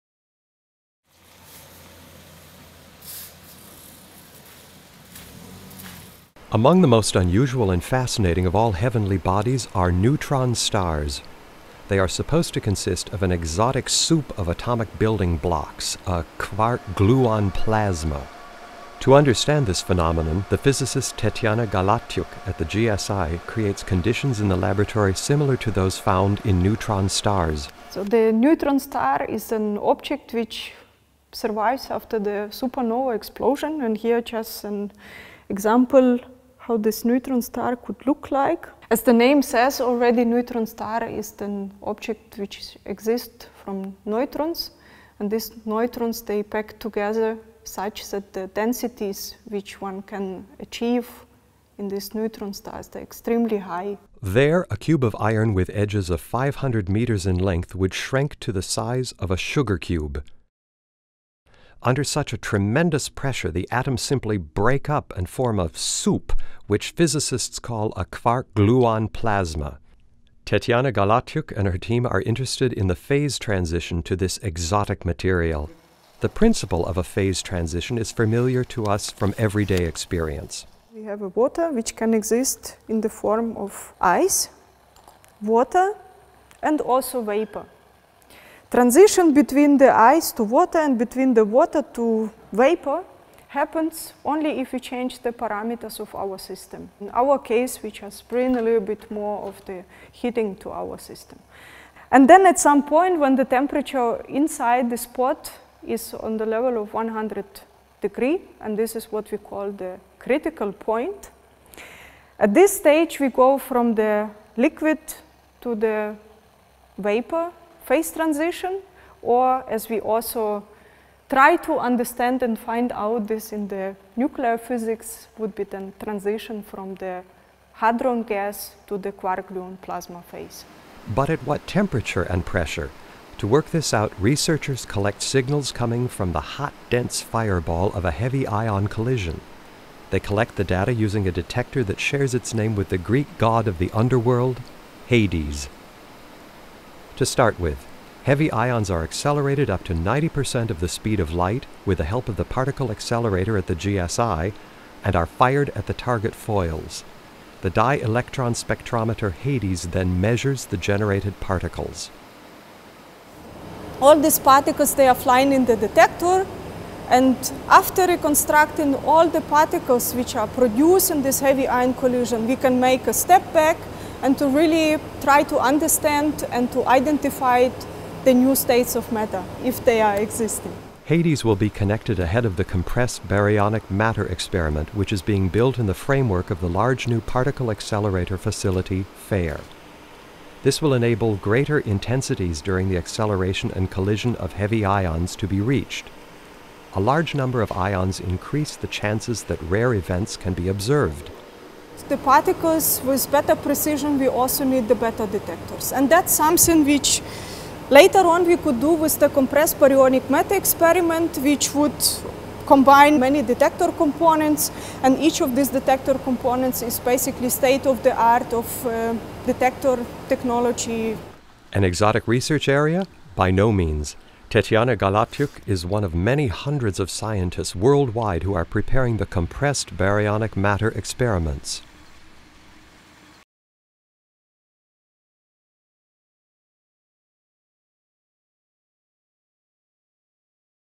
Neutron stars - A melting pot for atomic nuclei
CC-BY-ND
Neutron stars are extraordinary objects in the universe. Their density is so high that atoms melt within them and new states of matter arise. Tetyana Galatyuk and her colleagues try to simulate these conditions at the smallest scale using the FAIR particle accelerator and observe the results with the CBM detector. Thus, they not only learn more about neutron stars, but also about the innermost structure of matter.

Tetyana Galatyuk is head of a junior research group at GSI Helmholtz and the TU Darmstadt. With her group, she carries out experiments using HADES and CBM to explore compressed nuclear matter. Galatyuk studied physics in Ukraine, worked with the STAR detector at the RHIC accelerator facility in the U.S. and subsequently conducted research in Germany.